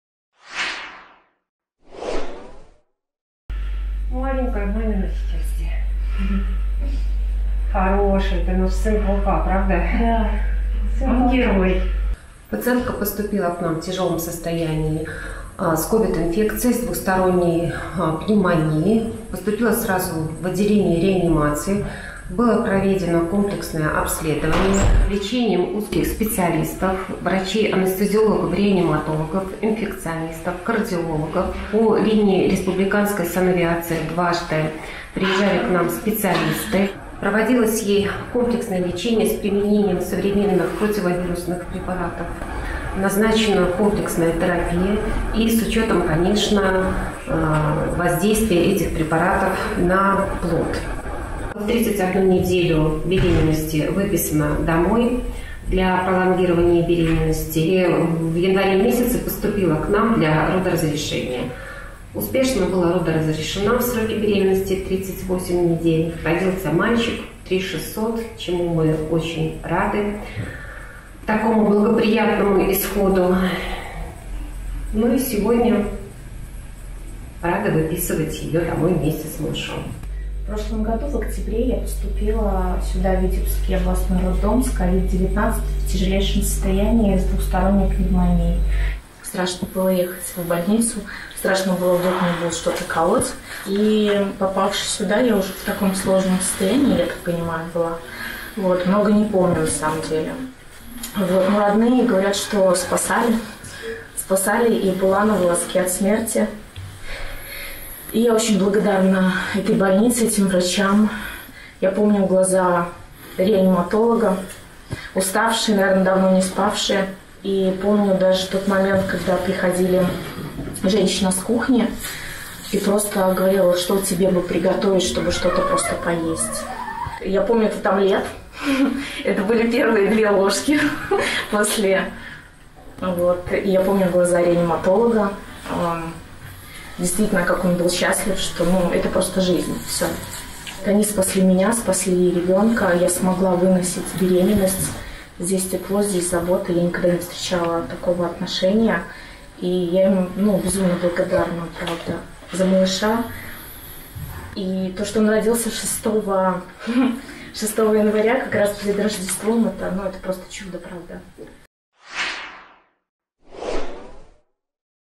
"Я помню глаза реаниматолога, как он был счастлив!" | Врачи 3 недели боролись за жизнь белоруски
Белоруска Елена Зинькевич перенесла коронавирус на втором триместре беременности. Три недели медики Витебского областного клинического роддома боролись за жизнь женщины и ее малыша в реанимации.

Мама и медики рассказали журналистам, как боролись за жизнь на протяжении трех непростых недель и о положительном исходе лечения. Подробности